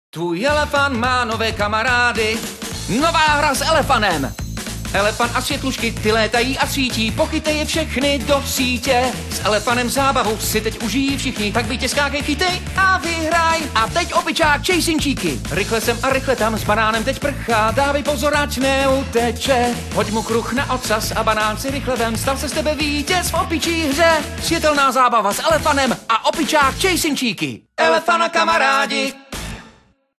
Elefun a Chasin Cheeky- společenské hry
Při společenské hře Elefun s fosforeskujícími motýlky je tvým úkolem pomoci sloníkovi pochytat světlušky. Tuto hru můžeš hrát i bez světla. Při hře Chasin Cheeky pobíhá opičák po pokoji a tvým úkolem je ho dohnat a házet na jeho ocas kroužky.